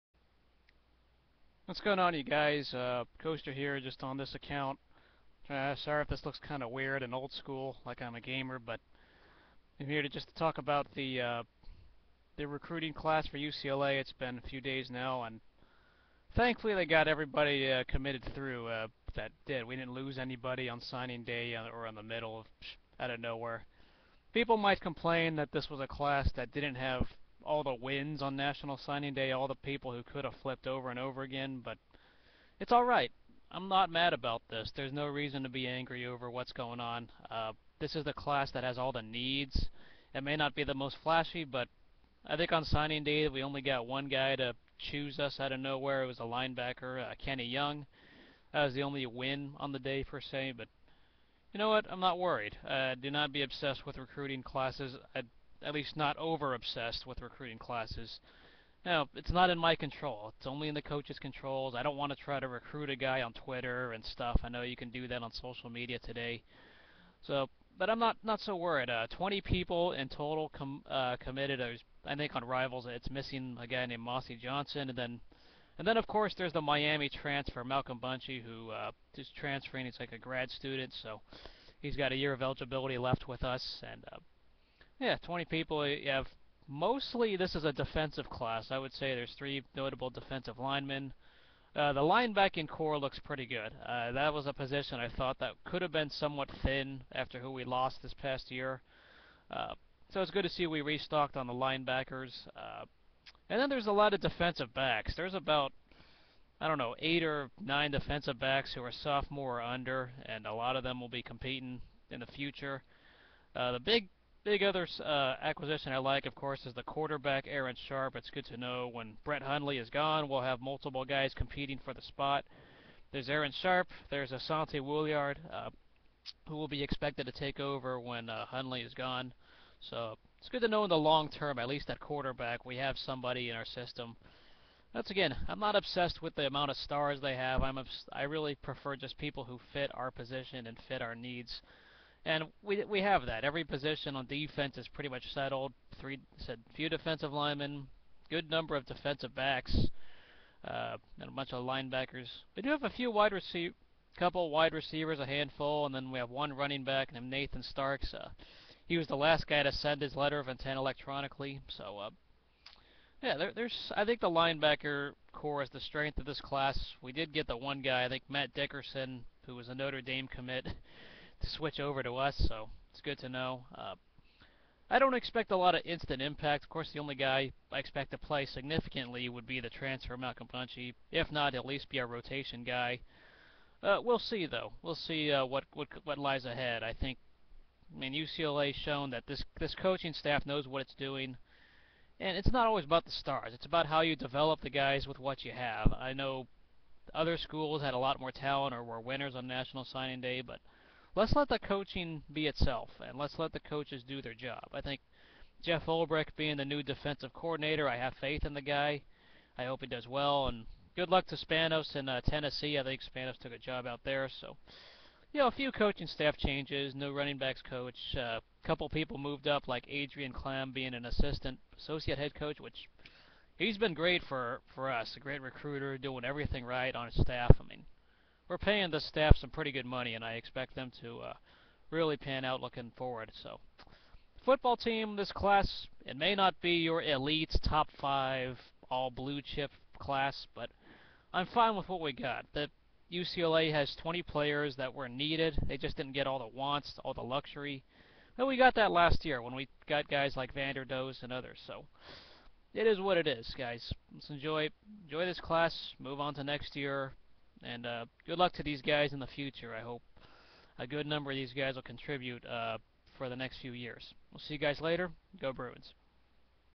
UCLA 2014 Recruiting Recap
A few days after National Signing day and the Bruins hauled in 20 players in this class.